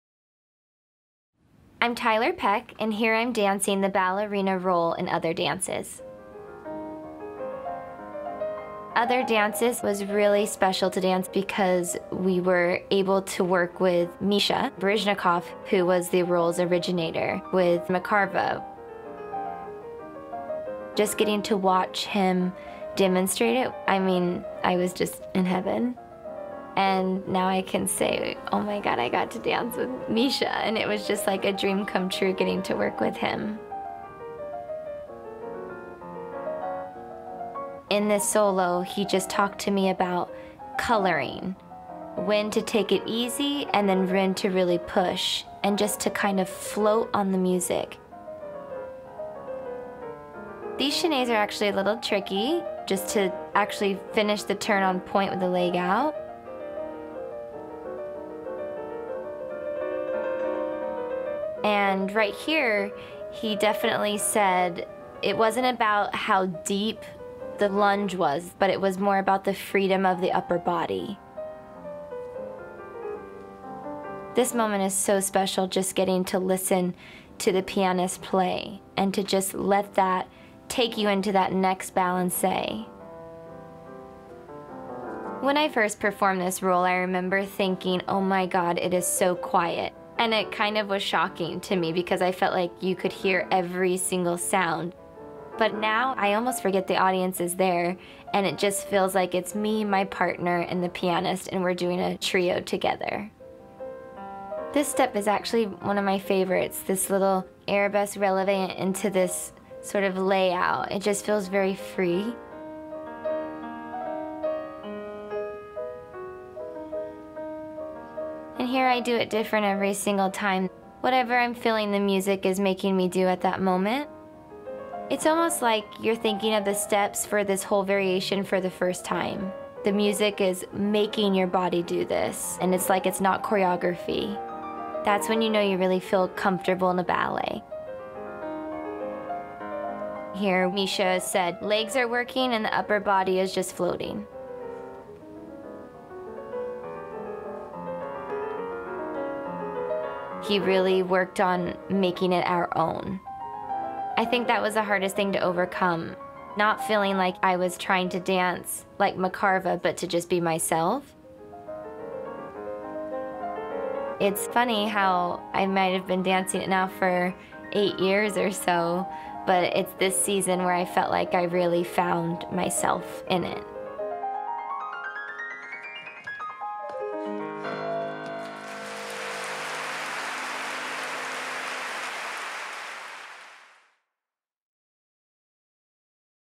NYC Ballet's Tiler Peck on Jerome Robbins' OTHER DANCES: Anatomy of a Dance
Hear from Tiler Peck about her “heavenly” experience learning how to float on the music in Jerome Robbins’ Other Dances, where she rehearsed the work with one of its original performers: the legendary Mikhail Baryshnikov.